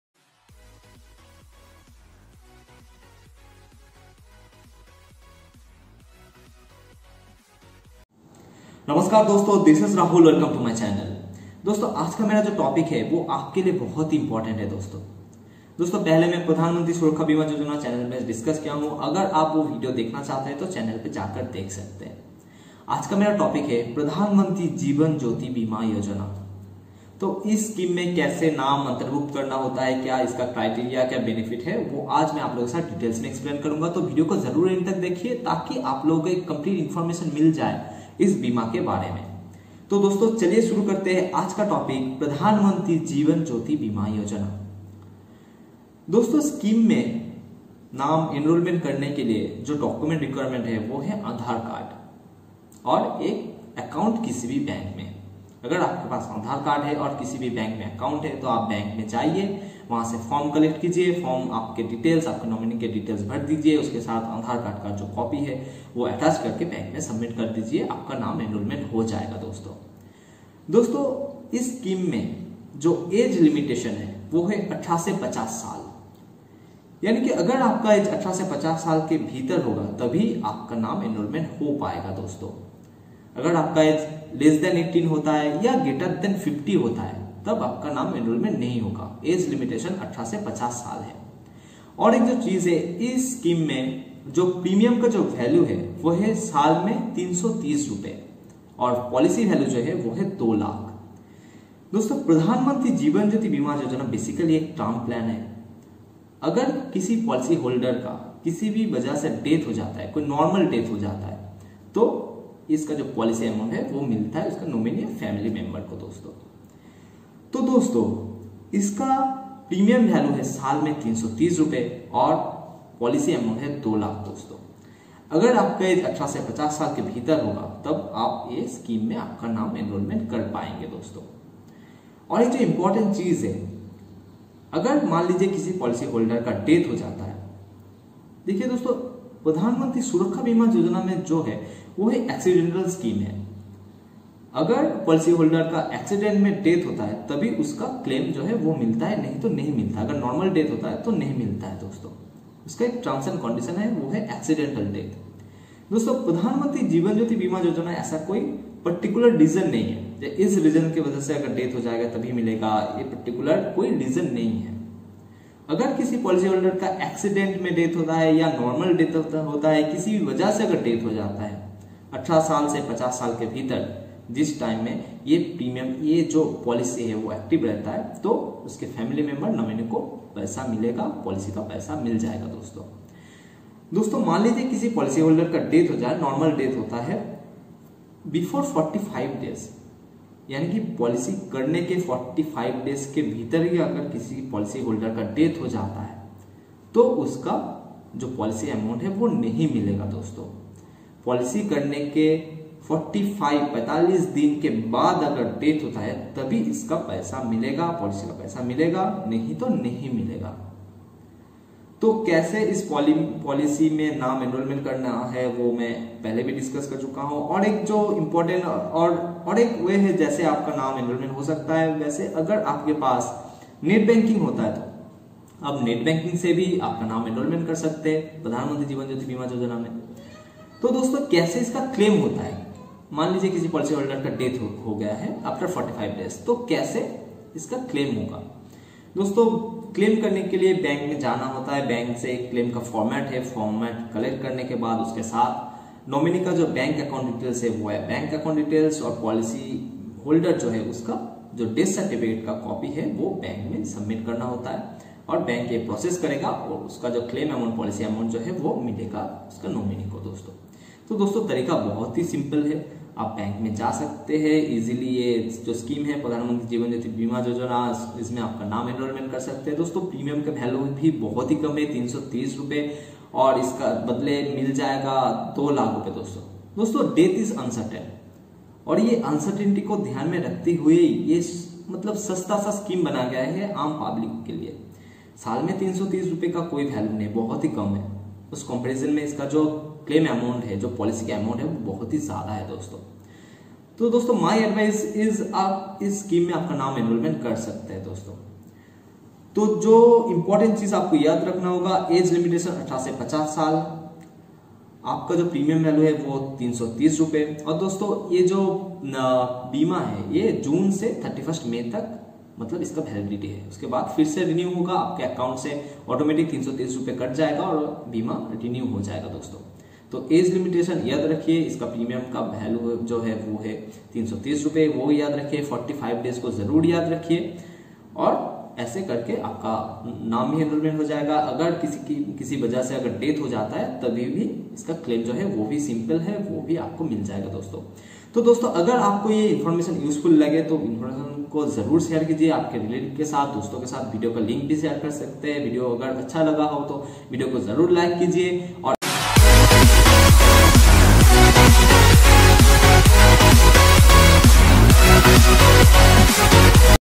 🔴 Pradhanmantri jeevan jyoti bima yojona | in hindi| details discussion| experts advice
What is pradhanmantri jeevan jyoti bima yojona details decussion in hindi. The criteria benefits all policy details explained in this video.